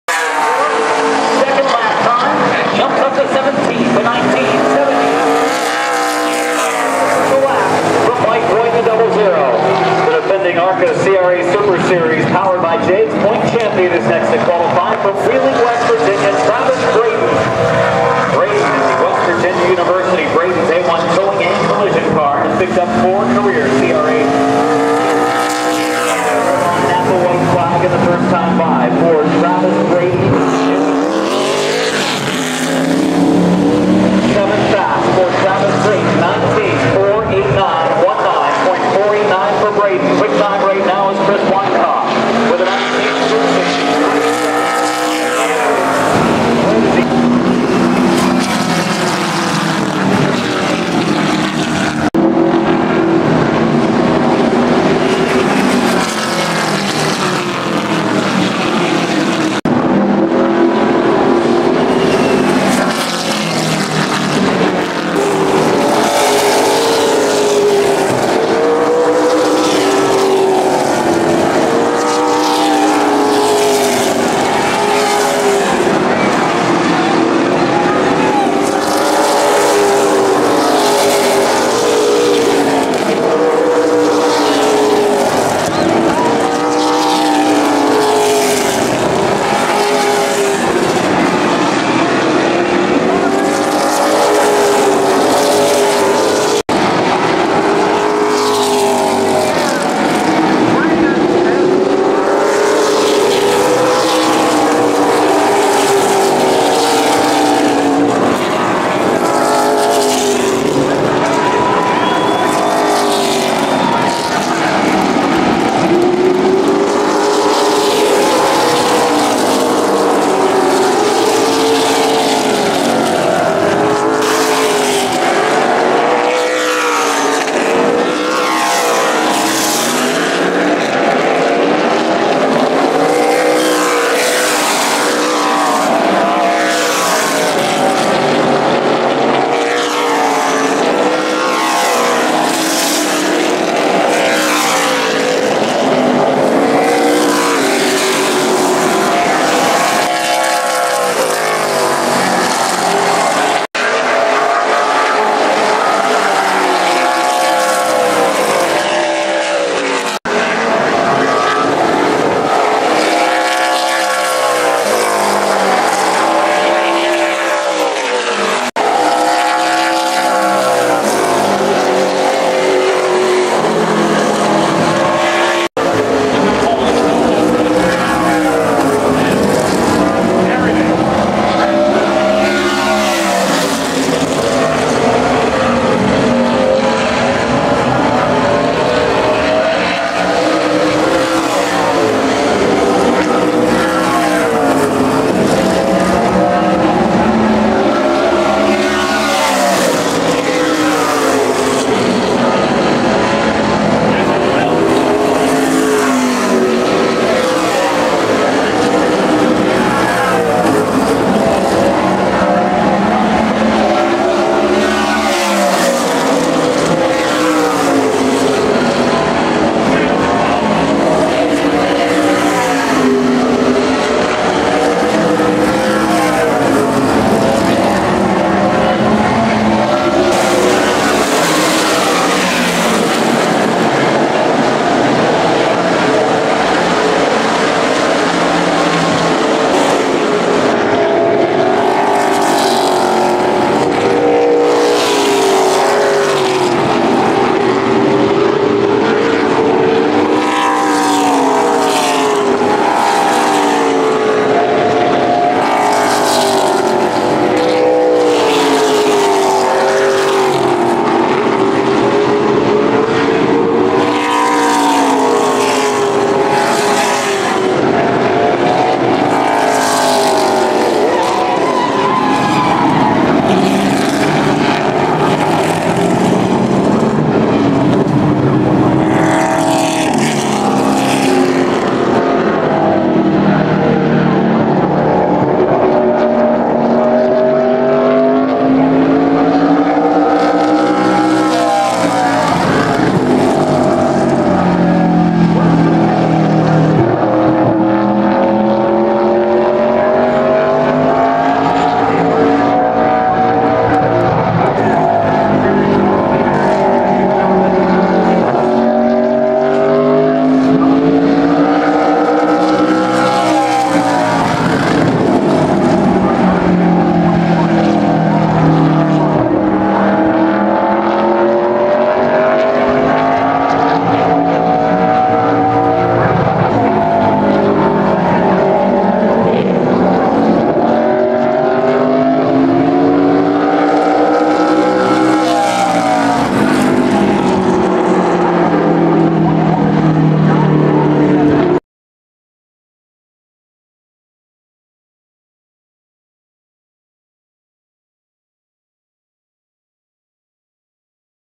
Travis Braden 2014 Highlights
A review showing various clips of 2 time and most recent ARCA/CRA Super Series champion Travis Braden racing in the series throughout the 2014 season.